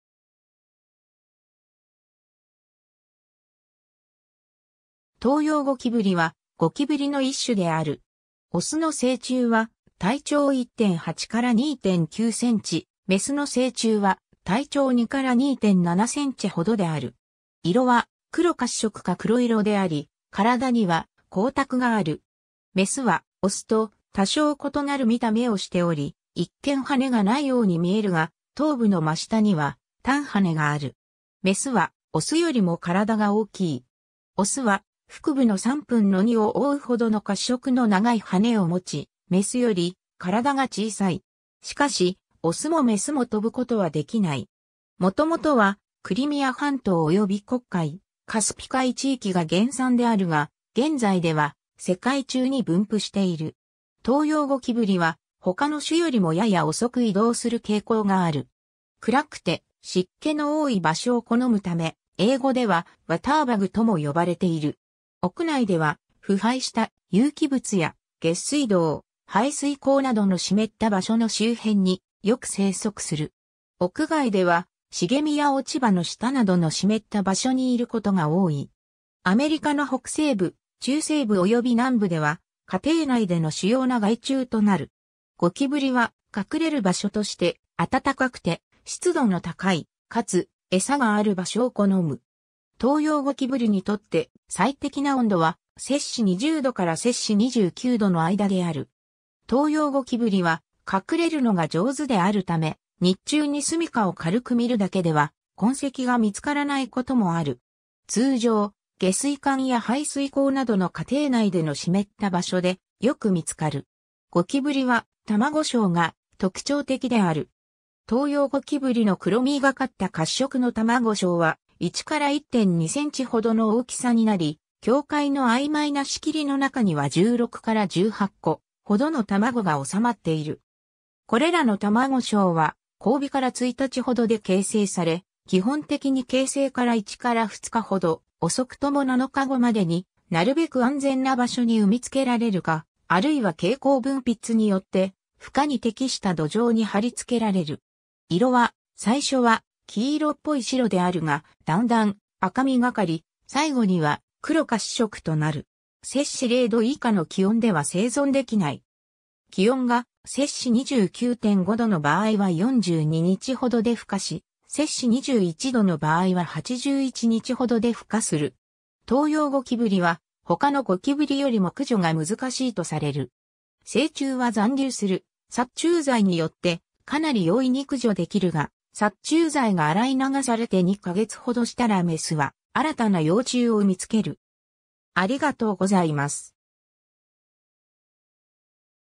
トウヨウゴキブリ
トウヨウゴキブリ（学名：Blatta orientalis）は、ゴキブリの一種である。
オスの成虫は体長1.8～2.9センチ、メスの成虫は体長2～2.7センチほどである。
色は黒褐色か黒色であり、体には光沢がある。
メスはオスと多少異なる見た目をしており、一見翅がないように見えるが、頭部の真下には短翅がある。
メスはオスよりも体が大きい。
オスは腹部の3分の2を覆うほどの褐色の長い翅を持ち、メスより体が小さい。
しかし、オスもメスも飛ぶことはできない。
もともとはクリミア半島および黒海、カスピ海地域が原産であるが、現在では世界中に分布している 。
 トウヨウゴキブリは他の種よりもやや遅く移動する傾向がある。
暗くて湿気の多い場所を好むため、英語では”waterbug”とも呼ばれている。
屋内では、腐敗した有機物や下水道、排水溝などの湿った場所の周辺によく生息する。
屋外では、繁みや落ち葉の下などの湿った場所にいることが多い。
アメリカの北西部、中西部および南部では家庭内での主要な害虫となる。
 ゴキブリは隠れる場所として、暖かくて湿度の高い、かつエサがある場所を好む。
トウヨウゴキブリにとって最適な温度は20℃から29℃の間である。
トウヨウゴキブリは隠れるのが上手であるため、日中にすみかを軽く見るだけでは痕跡が見つからないこともある。
通常、下水管や排水溝などの家庭内での湿った場所でよく見つかる。
 ゴキブリは卵鞘が特徴的である。
トウヨウゴキブリの黒みがかった褐色の卵鞘は、1～1.2センチほどの大きさになり、境界のあいまいな仕切りの中には16～18個ほどの卵が収まっている。
これらの卵鞘は交尾から1日ほどで形成され、基本的に形成から1～2日ほど、遅くとも7日後までに、なるべく安全な場所に産みつけられるか、あるいは経口分泌によって孵化に適した土壌に張りつけられる。
色は、最初は黄色っぽい白であるが、だんだん赤みがかり、最後には黒褐色となる。
0℃以下の気温では生存できない。
気温が29.5℃の場合は42日ほどで孵化し、21℃の場合は81日ほどで孵化する。
 トウヨウゴキブリは他のゴキブリよりも駆除が難しいとされる。
成虫は残留する殺虫剤によってかなり容易に駆除できるが、殺虫剤が洗い流されて2か月ほどしたらメスは新たな幼虫を産みつける。